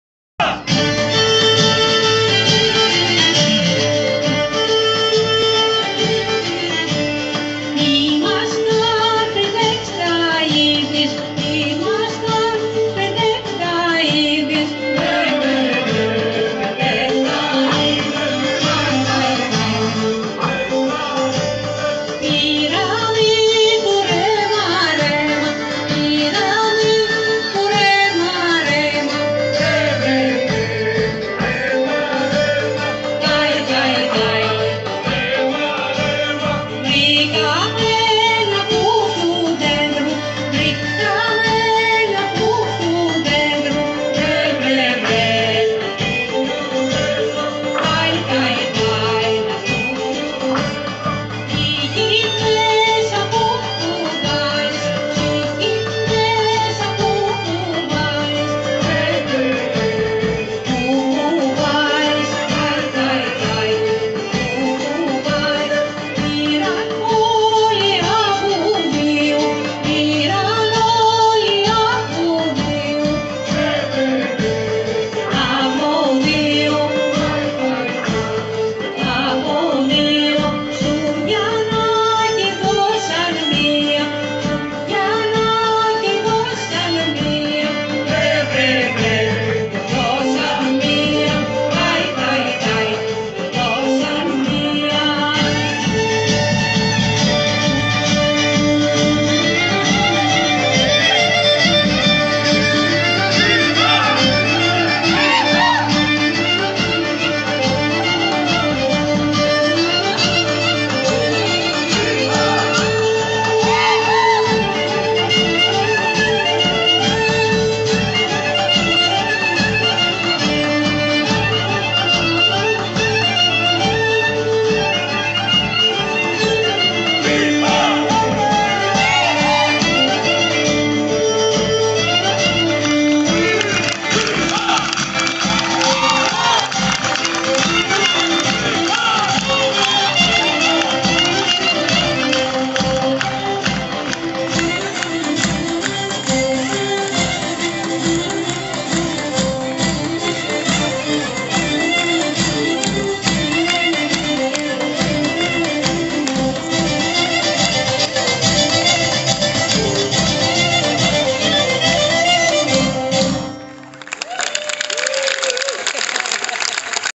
Dora Stratou Dance Theatre performing Zonaradikos
Zonaradikos from Thrace performed by the Dora Stratou Dance Theatre-- July 2022